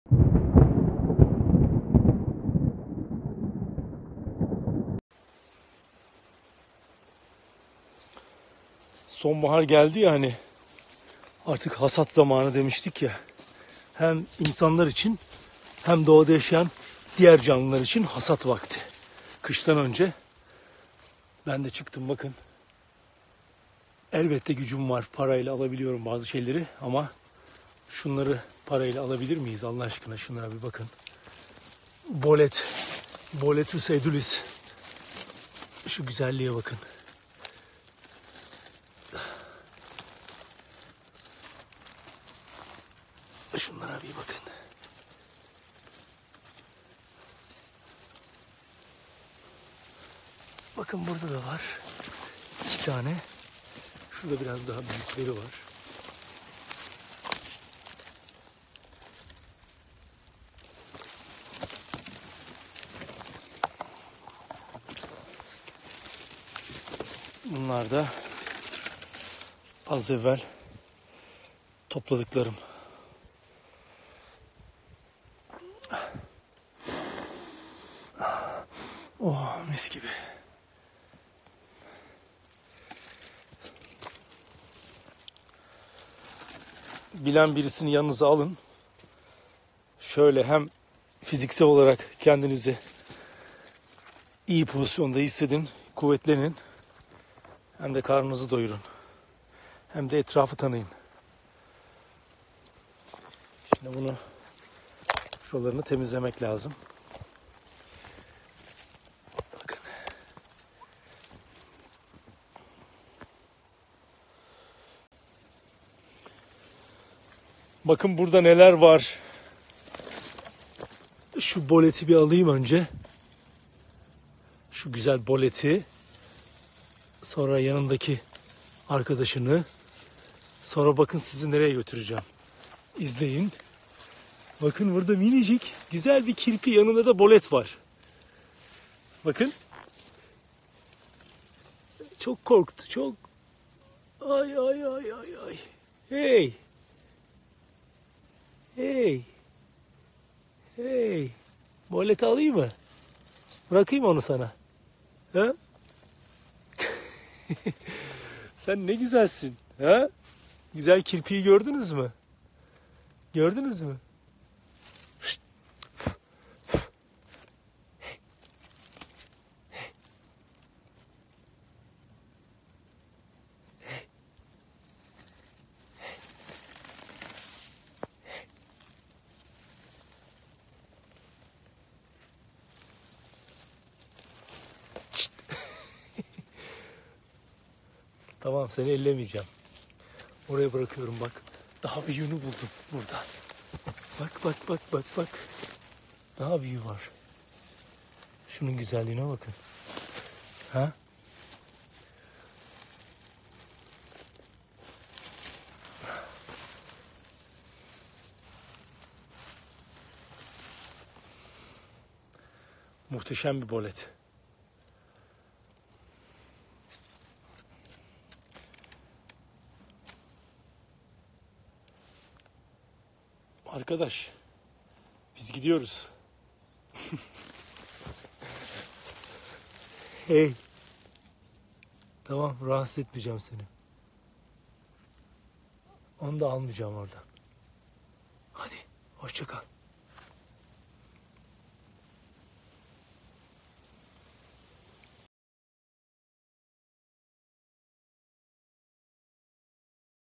Doğadaki İnsan - Serdar Kılıç Mantar Topluyor #2
Merhaba Şile Ağva Köyleri Takipçileri,

Severek izlediğimiz Doğadaki insan - Serdar Kılıç İnstagram hesabında ki canlı yayında toplamış olduğu mantarlar.